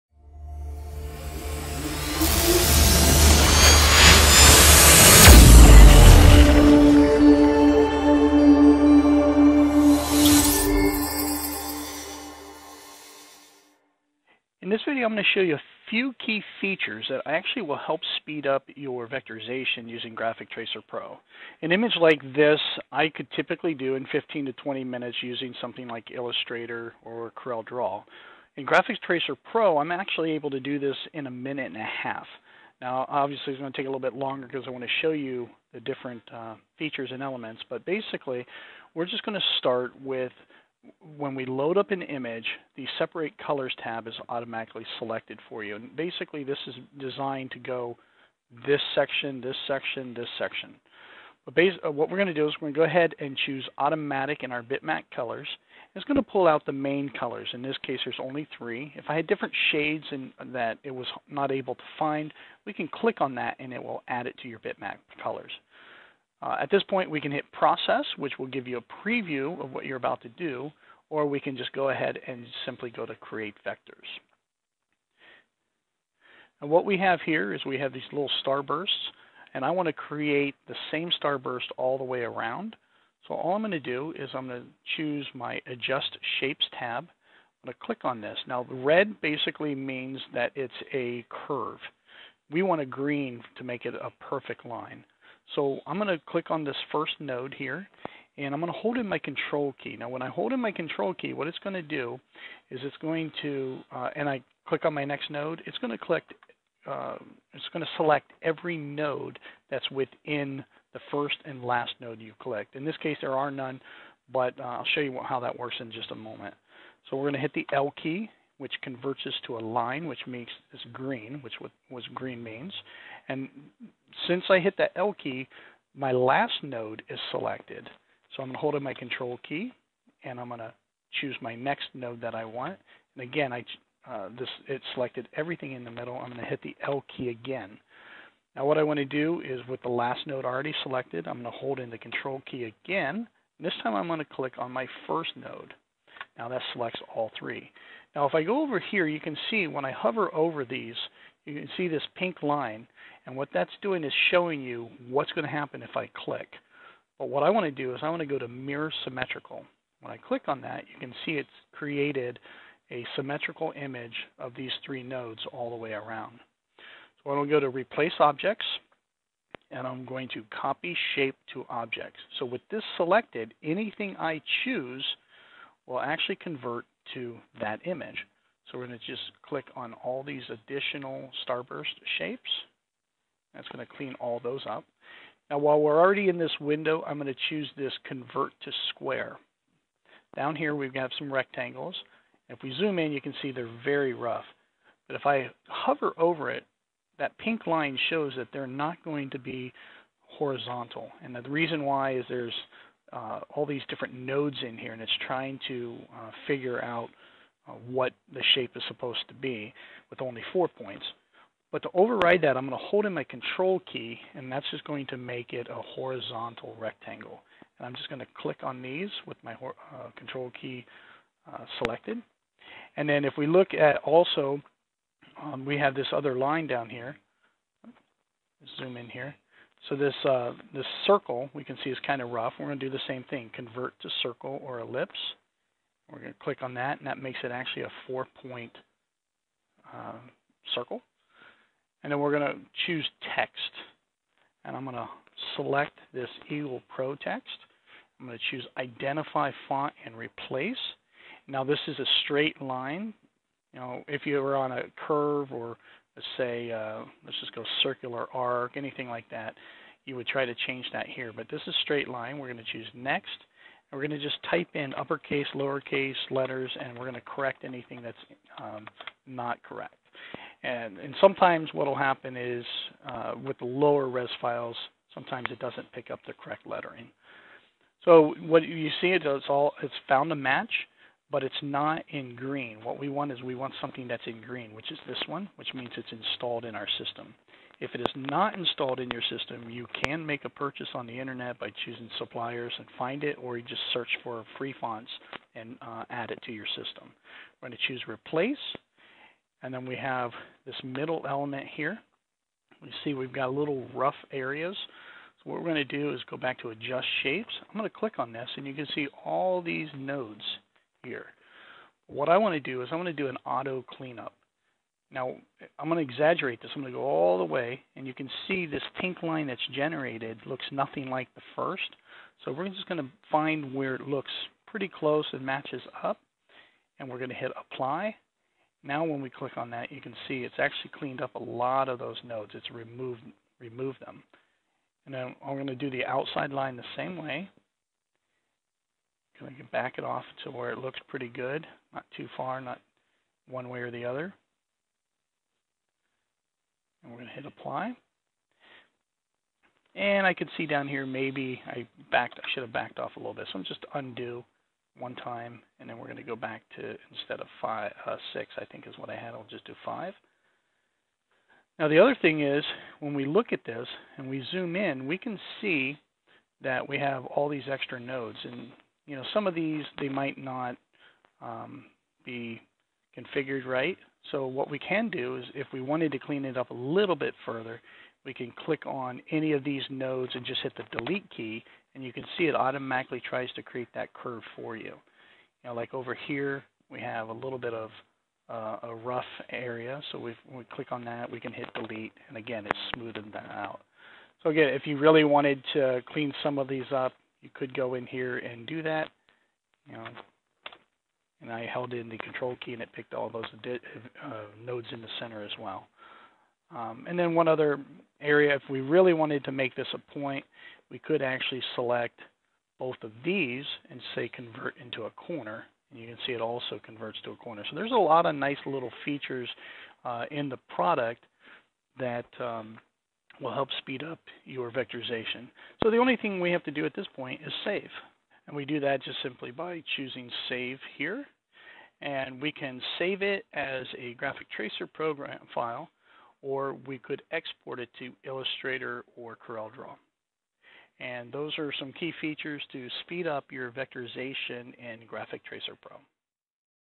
Using Graphic Tracer Pro
There are many tools you can use in Graphic Tracer Pro to speed up your tracing or vectorizing. This video shows some powerful tools to save time and money.

Use the coupon MaxBlack and get a year for $99 or if you commit to a month to month payment, your first month is free.